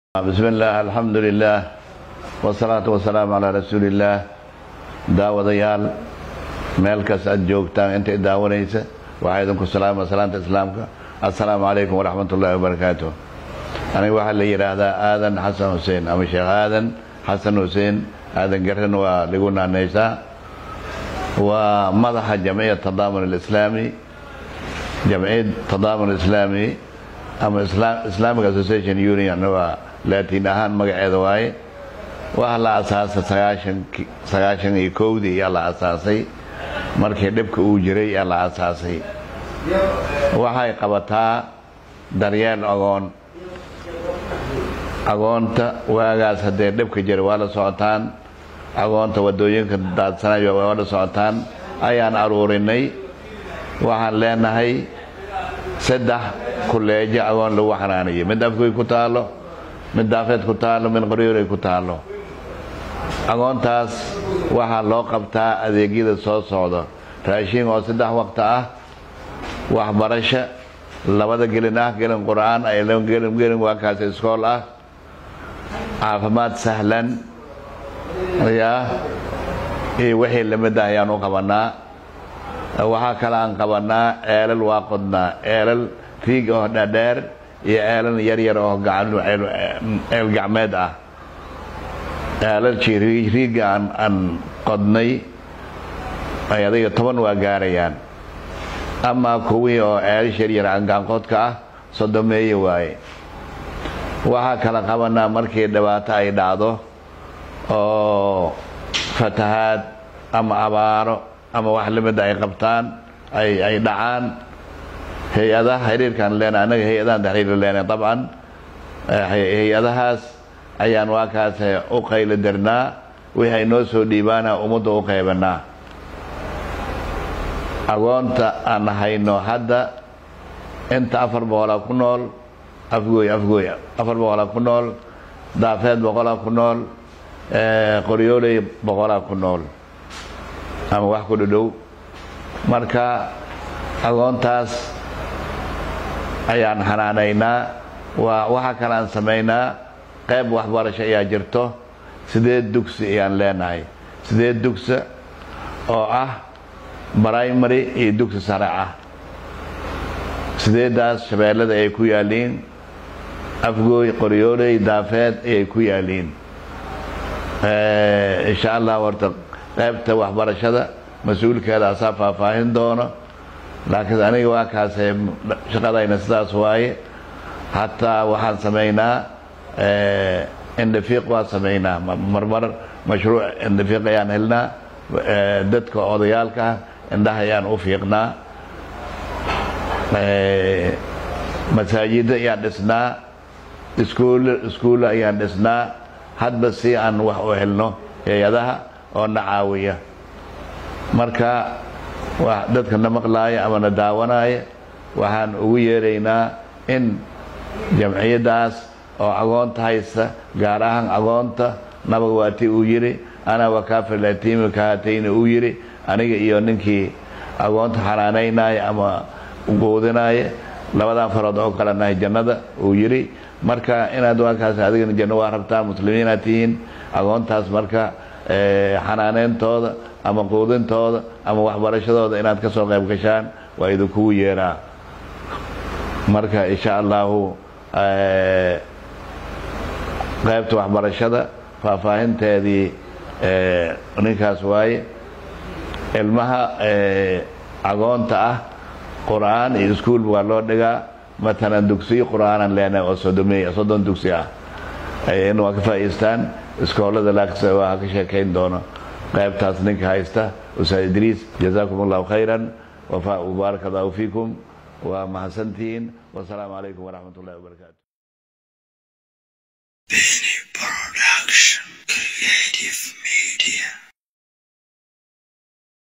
SHEIKH ADAN GARRE OO AH AABAHA WAXBATASHADA GOBOLKA SHABEELADA HOOSE OO KA HADLAY جمعية التضامن
SHEIKH ADAN GARRE OO AH AABAHA WAXBATASHADA GOBOLKA SHABEELADA HOOSE OO KA HADLAY XILIYADA AY SOO MARTAY جمعية التضامن الاسلامي OO AY WAX KU SOO BARTEEN DHALINYARADA UGU BADAN BEESHA GARRE IYO GUUD AHAAN SOMALIA.